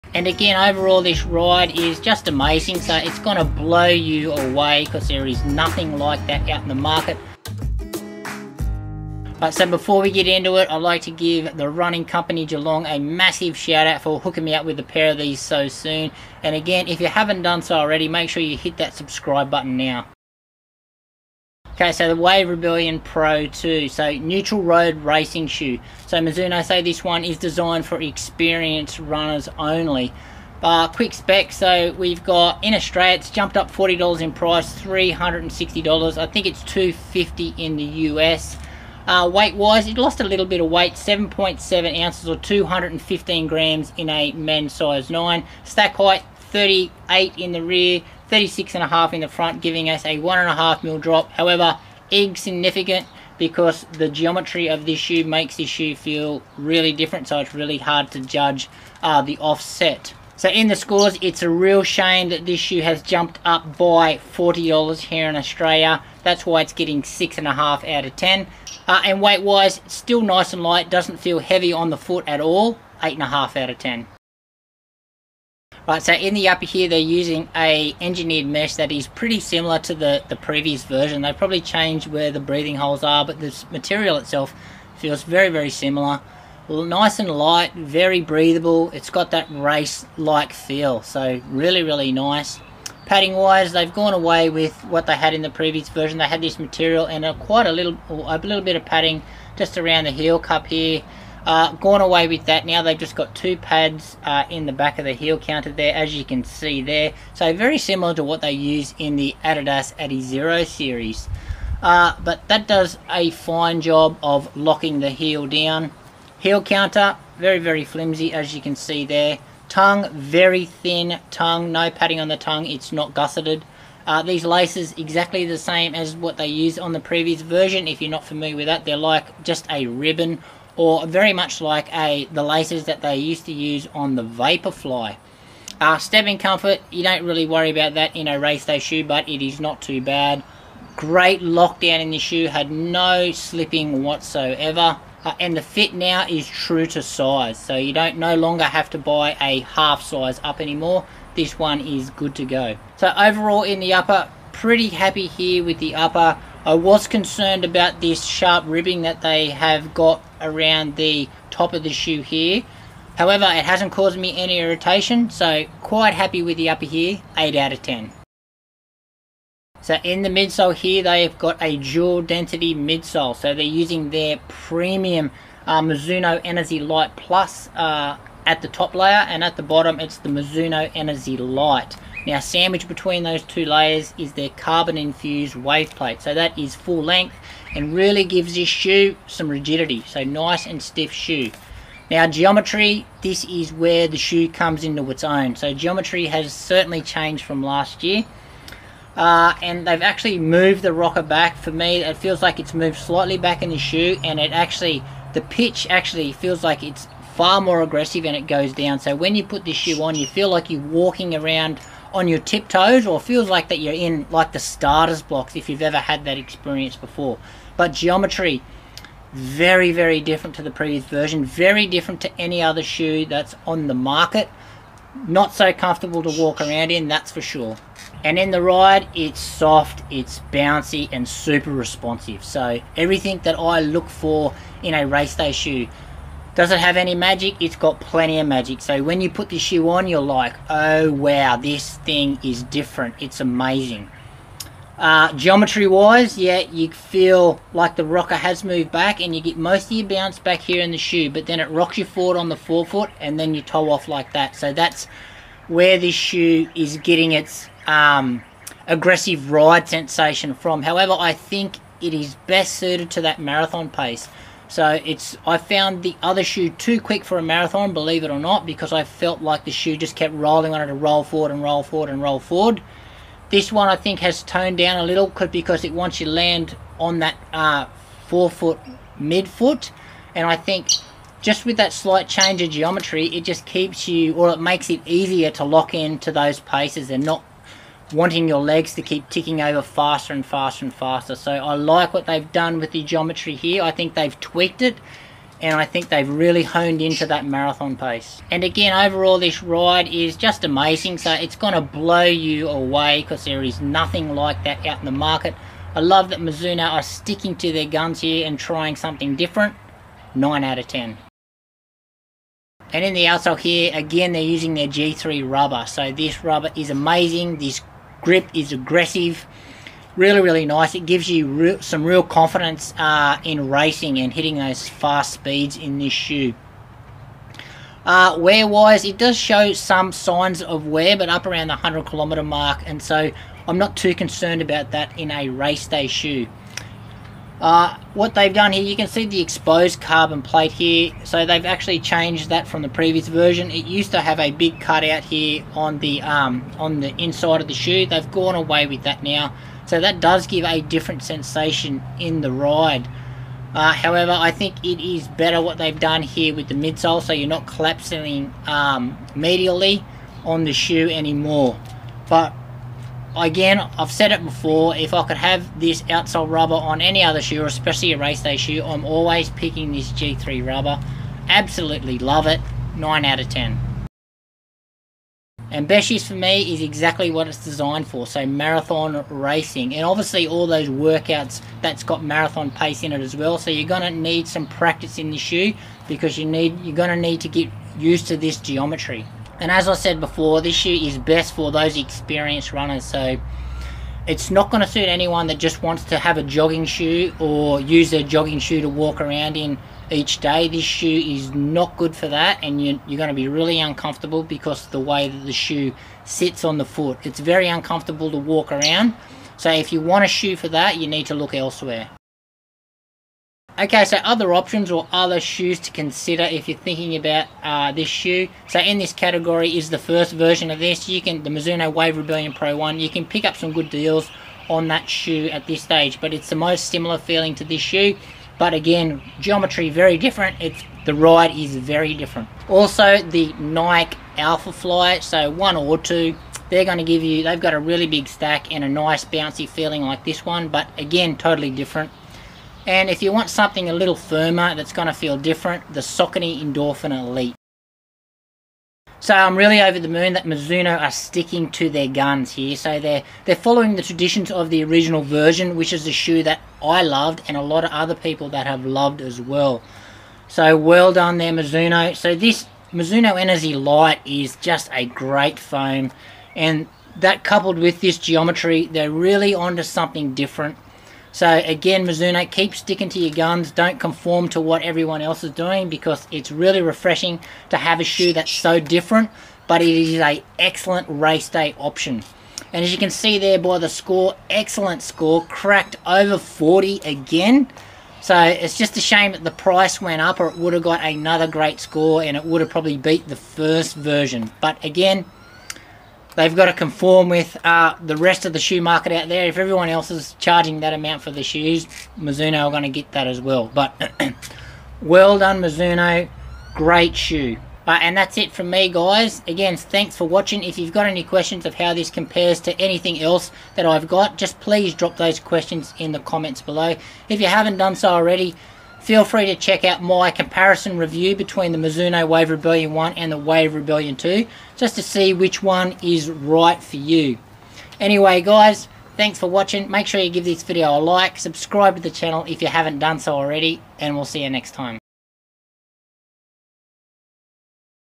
Mizuno Wave Rebellion Pro 2 - FULL REVIEW
This is Mizuno's second attempt at the marathon racing supershoe. The first version was a bold design with cutting edge technology we hadn't seen before.  The Rebellion Pro 1 certainly ticked all of my boxes and currently tops the RUN-ARE LEADERBOARD!

This video goes through the differences and discusses the new key performance features. Is it a move in the right direction or is it a step backwards?

Scores out of 10 are given in the categories of:
Price
Weight
Upper
Midsole
Outsole

Highest possible score is 50.

Contact the crew The Running Company Geelong to see if the Mizuno Wave Rebellion Pro 2 is right for you!